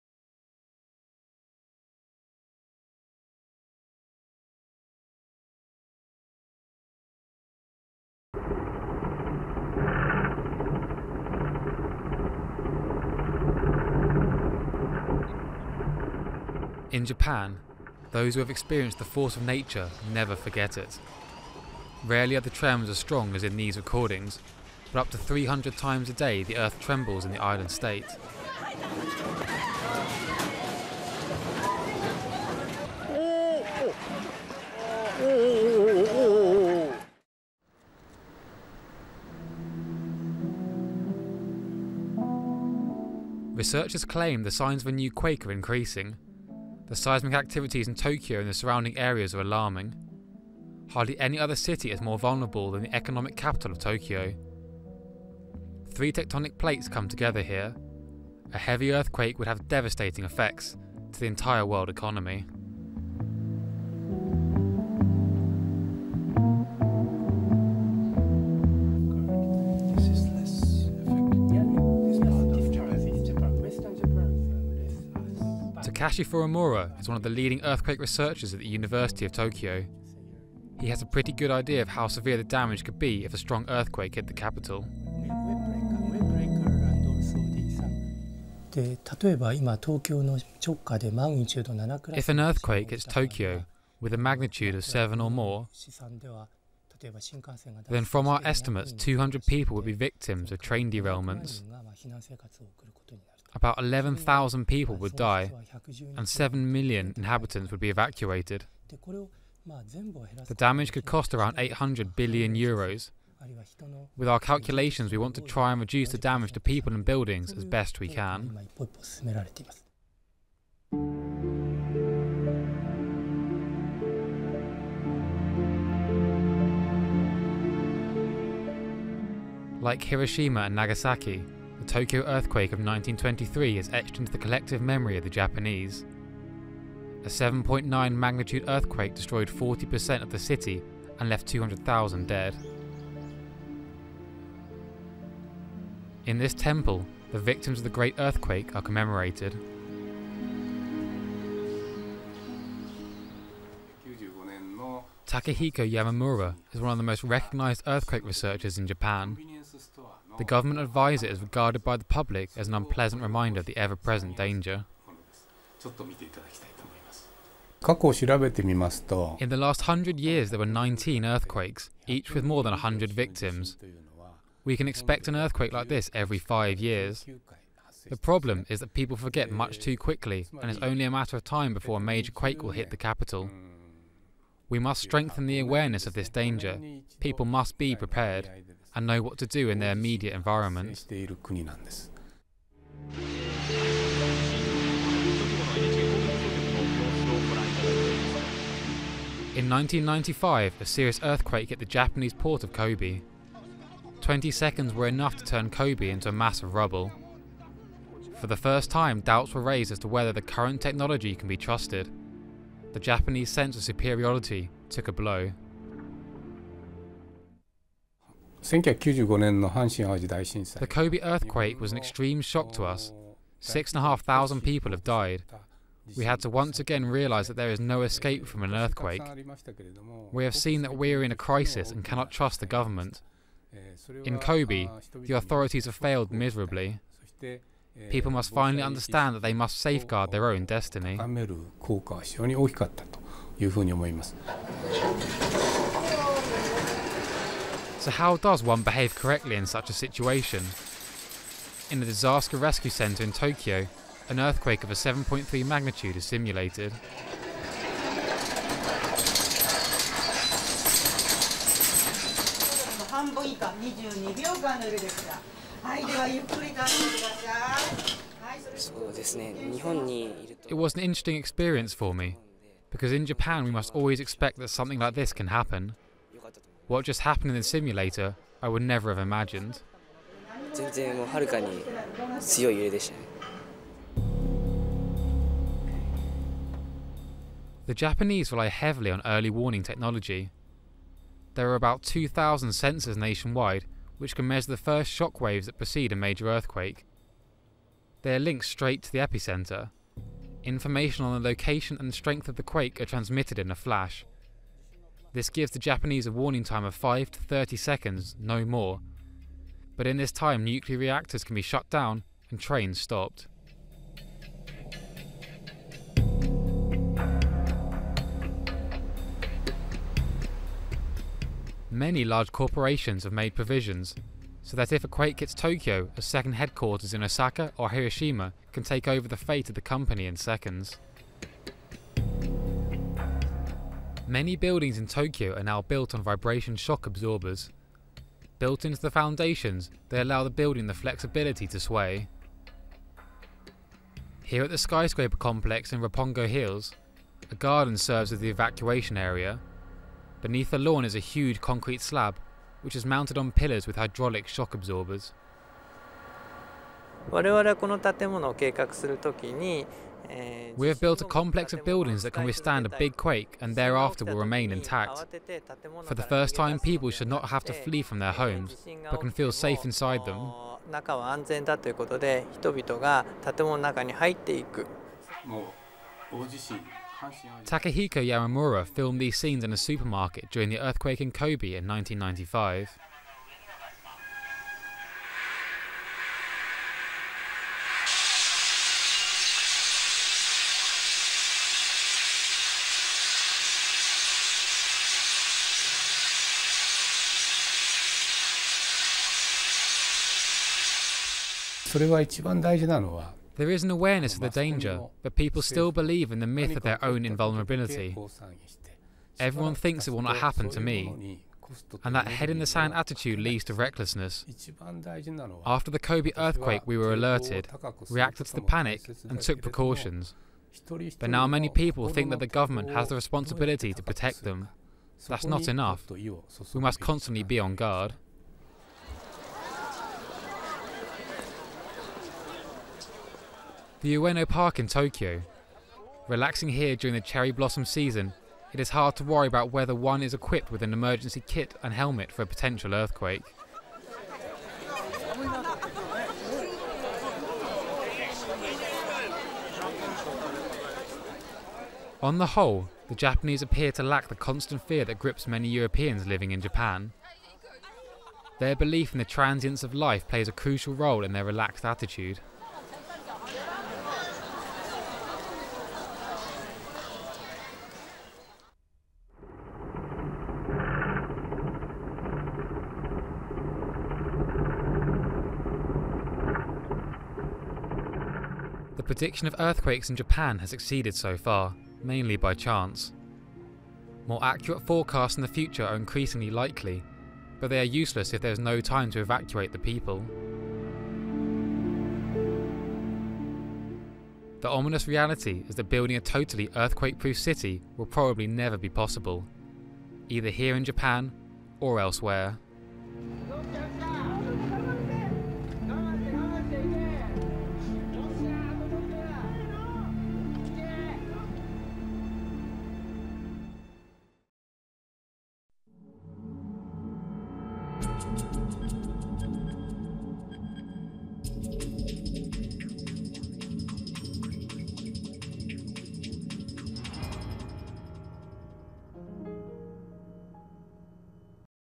Earthquake Insulation - Japan
April 2010

This report, released last year, gave a chilling prediction about the likelihood of a major earthquake hitting Japan. Now that its warning has come to bear, it offers a powerful background to the disaster.

Seismic activities in and around Tokyo have been alarming researchers for quite a while. In this report, researcher and government advisor Takehiko Yamamura insists a disaster is just around the corner for Japan. "We can expect an earthquake like this every five years...it's only a matter of time...". After Kobe was reduced to rubble in less than 20 seconds in 1995, the nation stepped up its earthquake technology. Urban planners in Tokyo have confidence in their latest shock-absorbing equipment; "For the first time people should not have to flee from their homes, but can feel safe inside them". Yet Yamamura argues that the biggest problem is the attitude of the Japanese; "people still believe in the myth of their own invulnerability. We must constantly be on guard". Haunting words in the face of the current crisis.